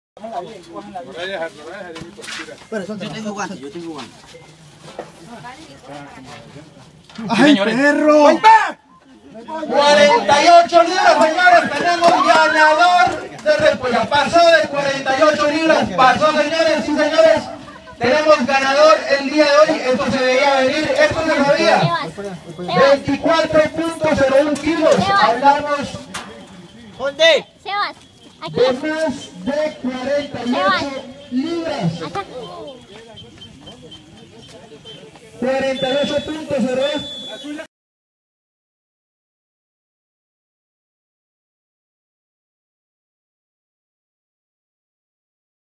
48.1 LIBRAS """$10.000.000 EN EFECTIVO""" German Conde
Pesando la Captura Ganadora de la Repolla "48.1 LIBRAS", la de los Primeros $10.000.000 En Efectivo!.. Felicitaciones!..